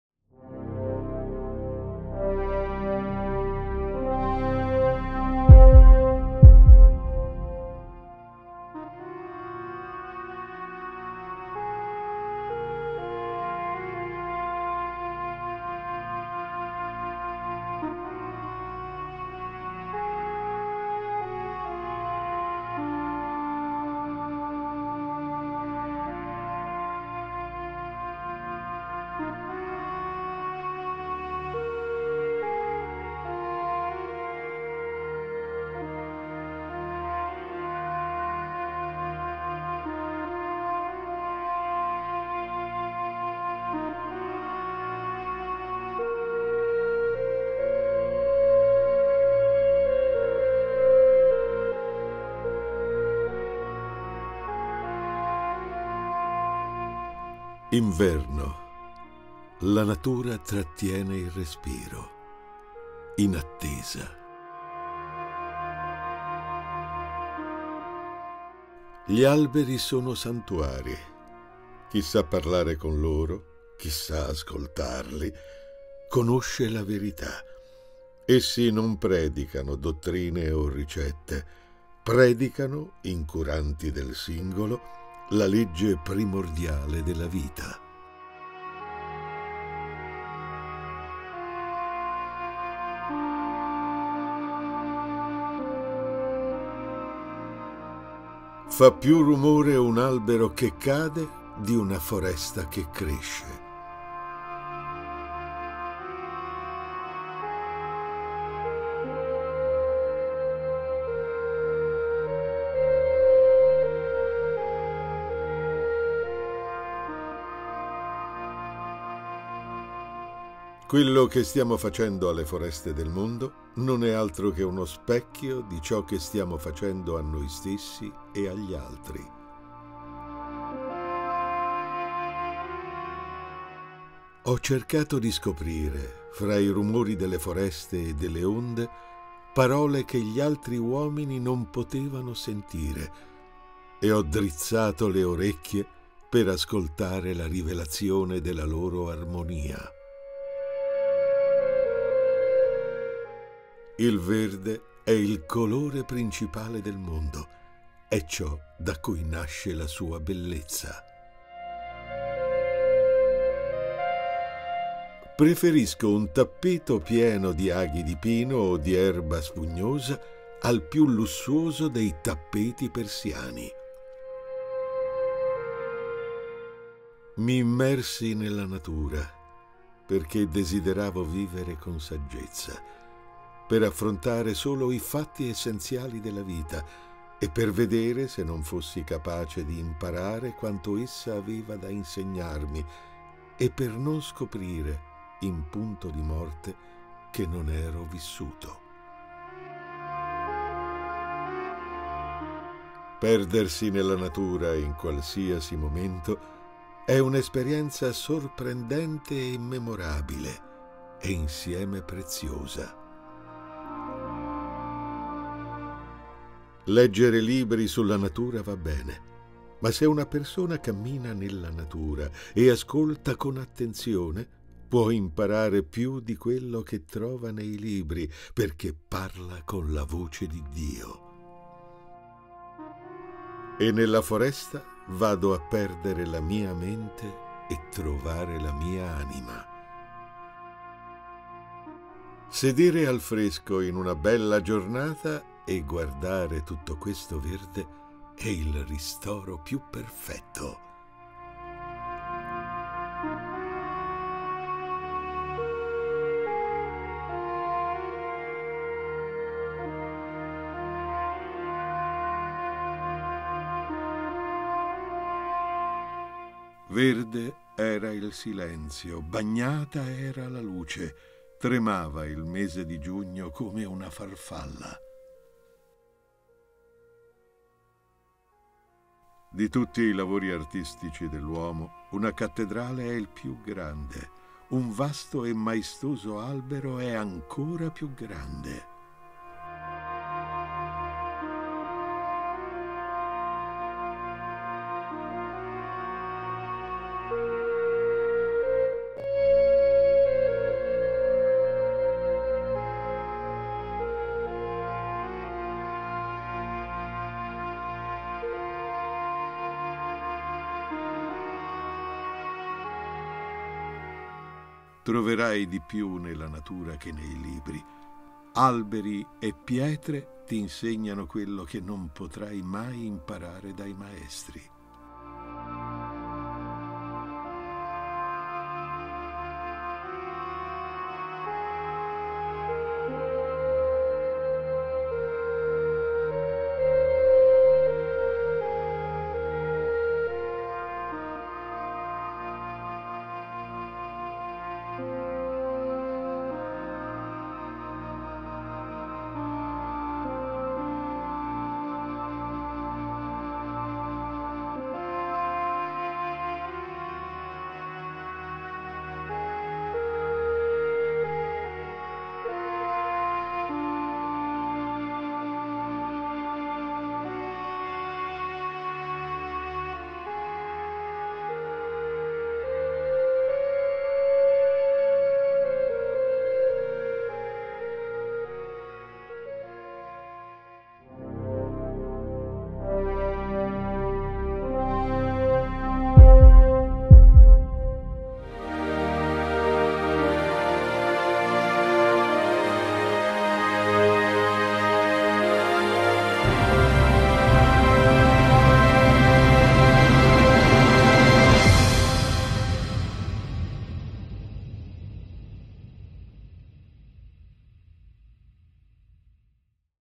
Un'oasi nel centro di Roma
Nel centro di Roma, tra le Terme di Caracalla e San Saba c'è un'oasi naturale di straordinaria bellezza, creata e curata da Giuseppe Marrocco con tanto lavoro, assoluta dedizione e amore.
A proposito, se non l’avete ancora fatto, solo per il piacere di saperne di più, venite a scoprire il nostro nuovo canale di video e podcast